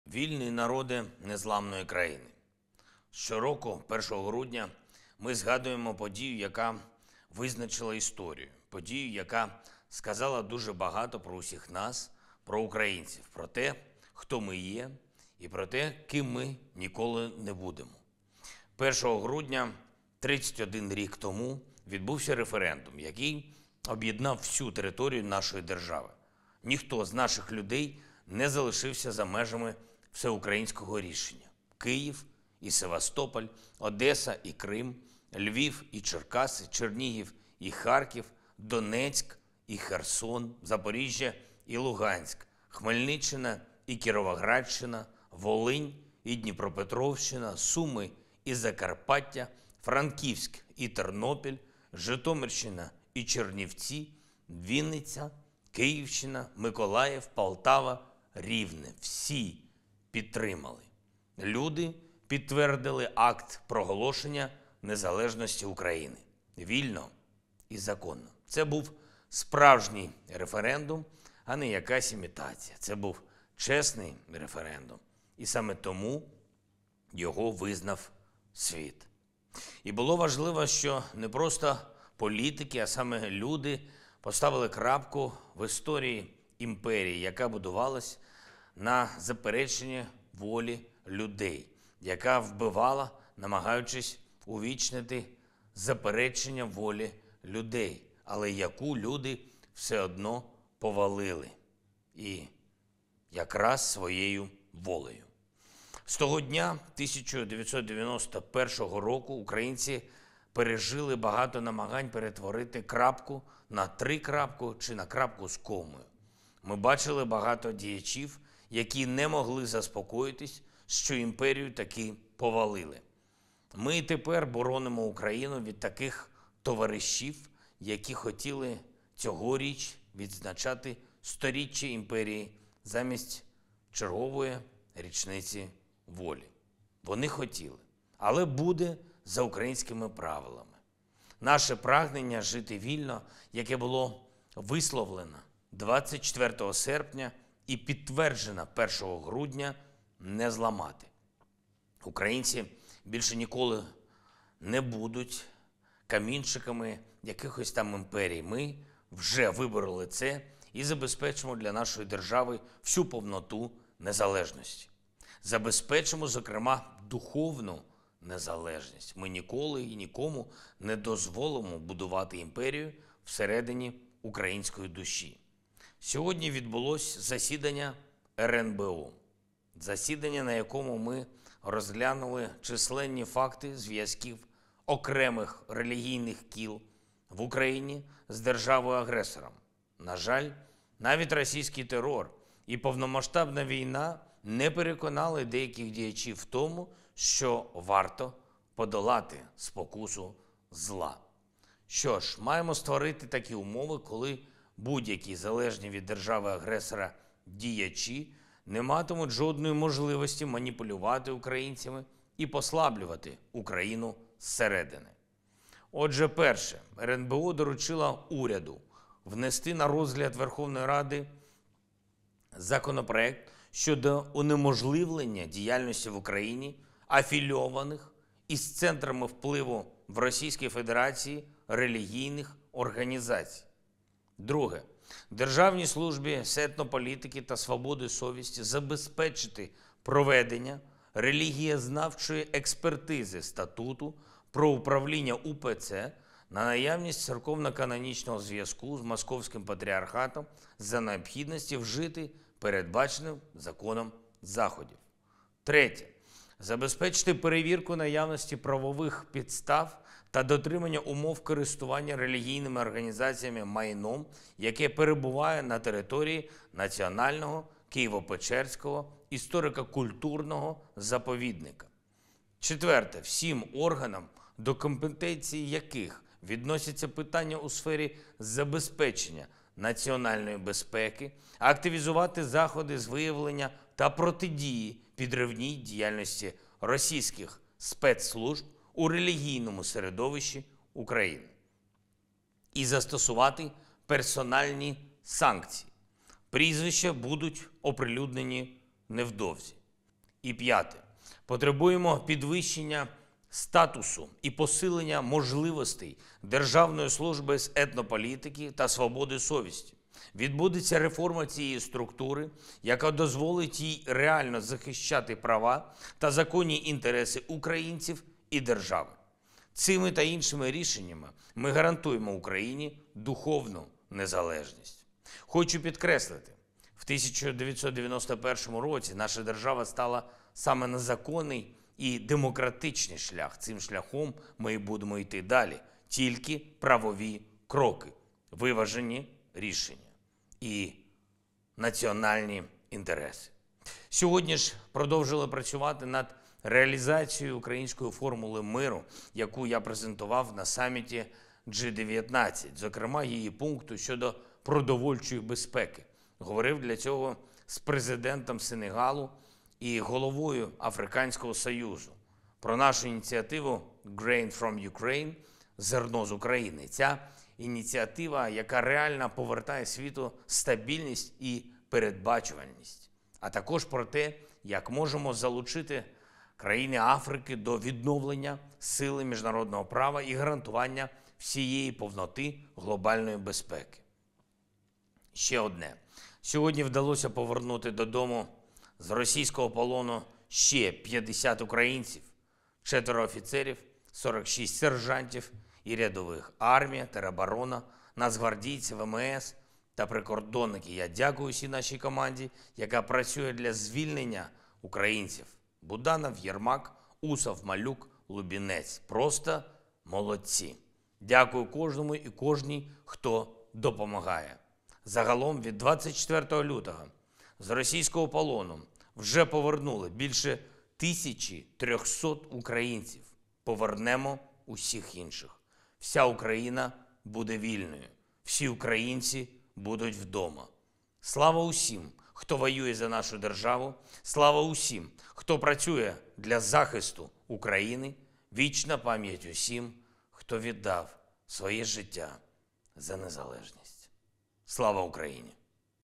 Звернення Президента Володимира Зеленського наприкінці 281-го дня повномасштабної війни
«Сьогодні відбулося засідання РНБО. Засідання, на якому ми розглянули численні факти звʼязків окремих релігійних кіл в Україні з державою-агресором. Маємо створити такі умови, за яких будь-які залежні від держави-агресора діячі не матимуть жодної можливості маніпулювати українцями і послаблювати Україну зсередини.
Отже, перше. РНБО доручила Уряду внести на розгляд Верховної Ради законопроект щодо унеможливлення діяльності в Україні афілійованих із центрами впливу в Російській Федерації релігійних організацій.

Друге. Державній службі з етнополітики та свободи совісті – забезпечити проведення релігієзнавчої експертизи Статуту про управління УПЦ на наявність церковно-канонічного зв’язку з Московським патріархатом і за необхідності вжити передбачених законом заходів.

Третє. Забезпечити перевірку наявності правових підстав і дотримання умов користування релігійними організаціями майном, яке перебуває на території Національного Києво-Печерського історико-культурного заповідника.

Четверте. Усім органам, до компетенції яких належать питання у сфері забезпечення національної безпеки, активізувати заходи з виявлення та протидії підривній діяльності російських спецслужб у релігійному середовищі України. І застосувати персональні санкції – прізвища будуть оприлюднені невдовзі.

І пʼяте. Потребуємо підвищення статусу й посилення можливостей Державної служби з етнополітики та свободи совісті. Відбудеться реформа цієї структури, яка дасть їй змогу реально захищати права та законні інтереси українців і держави. Цими й іншими рішенням ми гарантуємо Україні духовну незалежність».